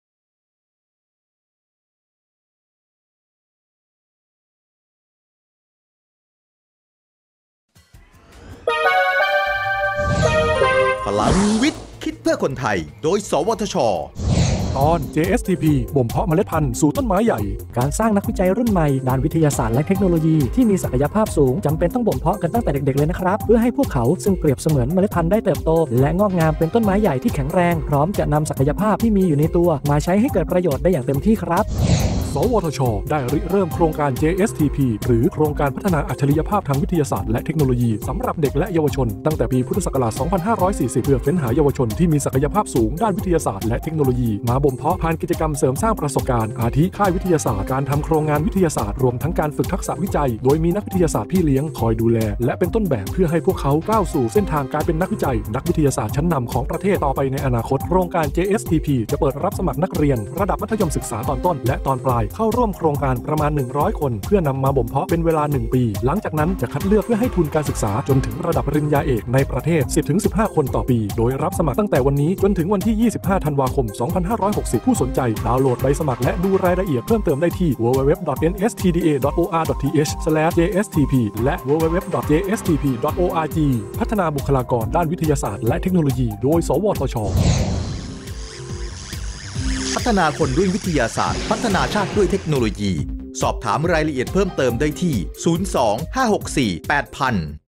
พลังวิทย์ คิดเพื่อคนไทย ตอน JSTP…บ่มเพาะเมล็ดพันธุ์สู่ต้นไม้ใหญ่
สวทช. ได้ริเริ่มโครงการ JSTP หรือ โครงการพัฒนาอัจฉริยภาพทางวิทยาศาสตร์และเทคโนโลยีสำหรับเด็กและเยาวชน ตั้งแต่ปี พ.ศ. 2540 เพื่อเฟ้นหาเยาวชนที่มีศักยภาพสูงด้านวิทยาศาสตร์และเทคโนโลยี มาบ่มเพาะ ผ่านกิจกรรมเสริมสร้างประสบการณ์ อาทิ ค่ายวิทยาศาสตร์ การทำโครงงานวิทยาศาสตร์ รวมทั้งการฝึกทักษะวิจัย โดยมีนักวิทยาศาสตร์พี่เลี้ยงคอยดูแล และเป็นต้นแบบเพื่อให้พวกเขาก้าวเข้าสู่เส้นทางการเป็นนักวิจัยนักวิทยาศาสตร์ชั้นนำของประเทศต่อไปในอนาคต

โครงการ JSTP จะเปิดรับสมัครนักเรียนระดับมัธยมศึกษาตอนต้นและตอนปลาย เข้าร่วมโครงการ ประมาณ 100 คน เพื่อนำมาบ่มเพาะเป็นเวลา 1 ปี หลังจากนั้นจะคัดเลือกเพื่อให้ทุนการศึกษาจนถึงระดับปริญญาเอก (ในประเทศ) 10-15 คน/ปี โดยรับสมัครตั้งแต่วันนี้ จนถึงวันที่ 25 ธันวาคม 2560 ผู้สนใจ ดาวน์โหลดใบสมัครและดูรายละเอียดเพิ่มเติมได้ที่ http// และ